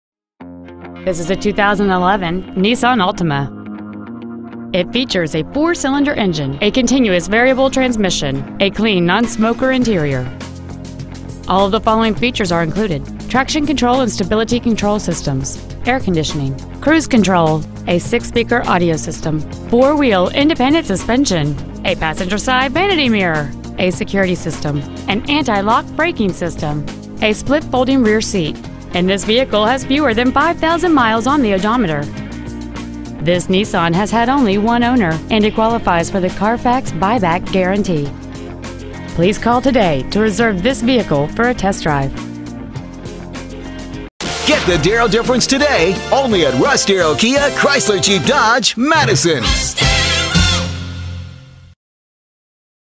2011 Altima Madison WI
Year : 2011
 Make :
 Model : Altima
 Engine : 2.50
 Trans . : CVT with Xtronic
 Exterior : Winter Frost Pearl
 Miles : 4,361
 Interior : Blond
 Stock : PCJM1893

 3502 Lancaster Dr.

 2011       Altima Madison WI